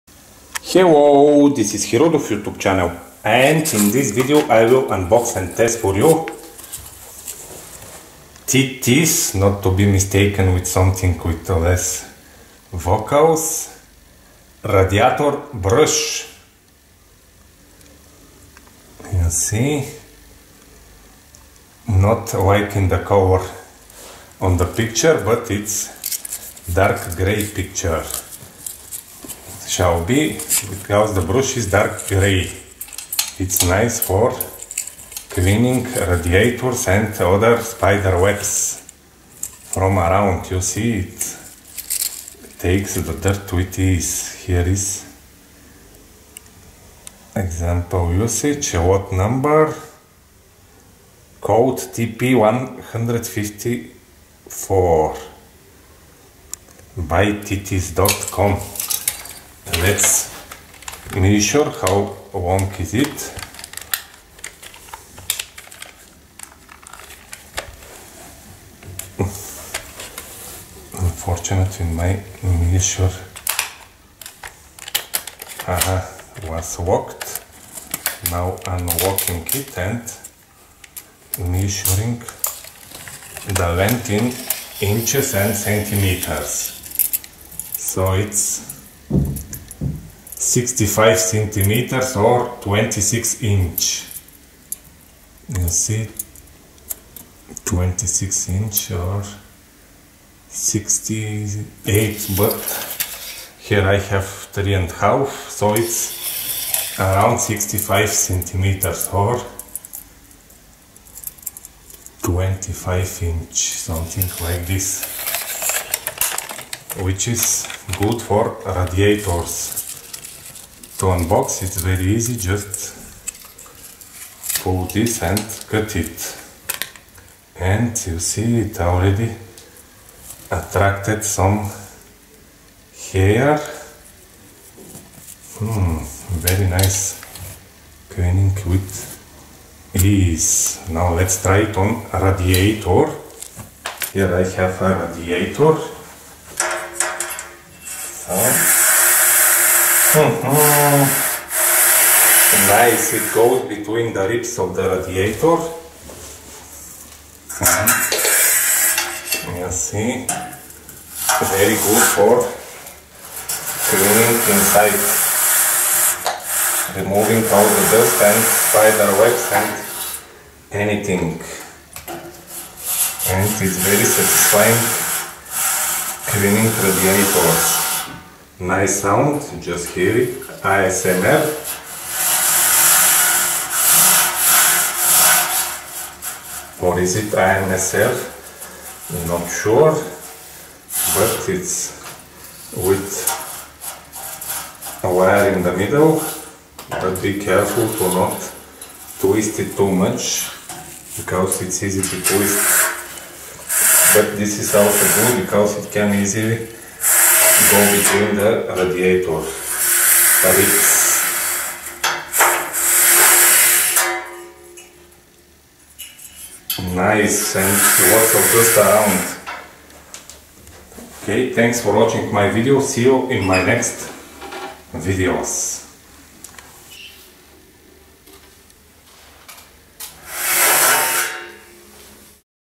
Titiz Plastic Radiator Brush Unboxing and Test
Unboxing and test of Titiz Plastic Radiator Brush. After unboxing the brush was tested on heating radiator.